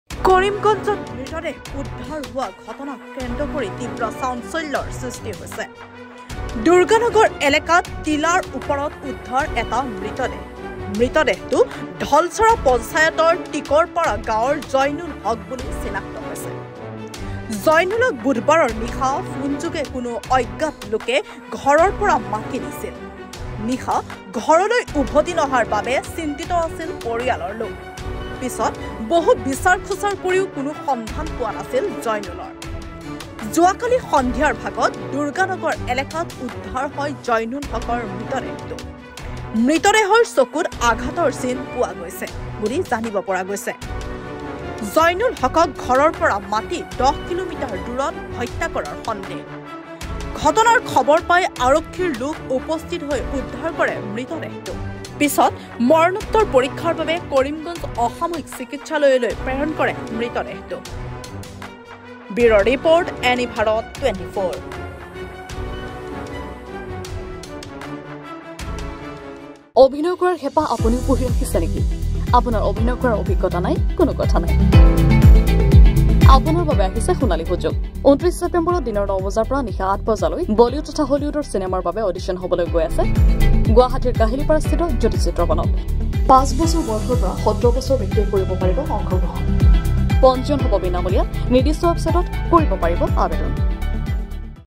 কৰিমগঞ্জৰ দুৰ্গানগৰত চাঞ্চল্যকৰ ঘটনা
এটা মৃতদেহ উদ্ধাৰক লৈ এই চাঞ্চল্য